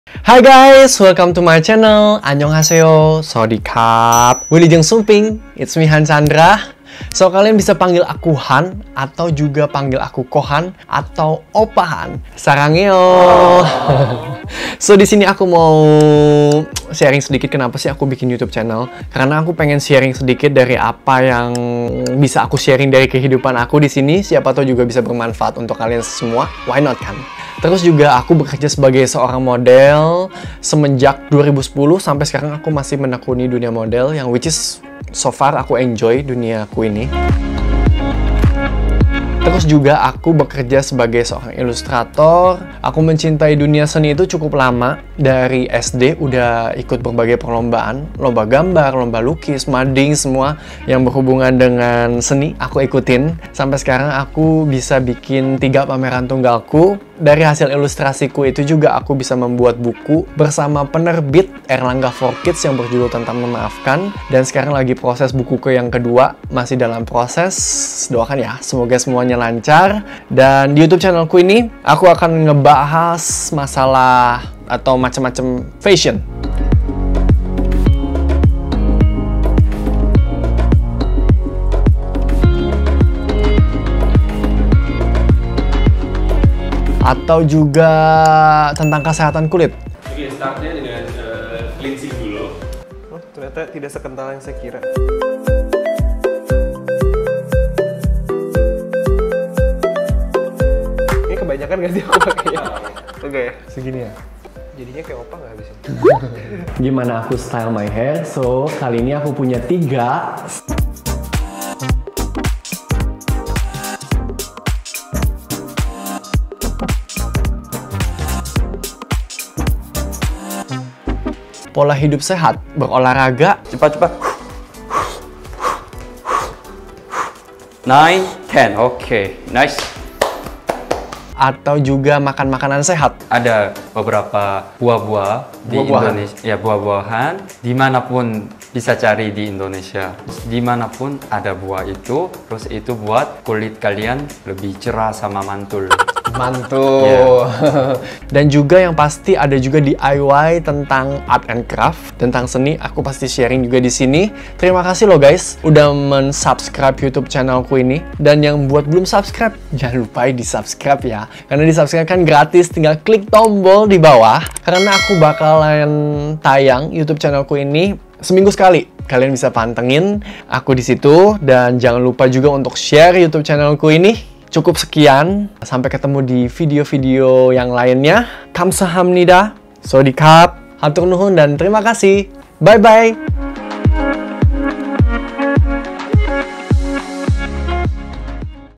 HAN CHANDRA YOUTUBE TRAILER
Hi, it's me Han Chandra, di Youtube Channel ini aku akan share seputar kehidupan modeling aku, Men's Grooming, Fashion & Lifestyle, serta Art & Craft juga!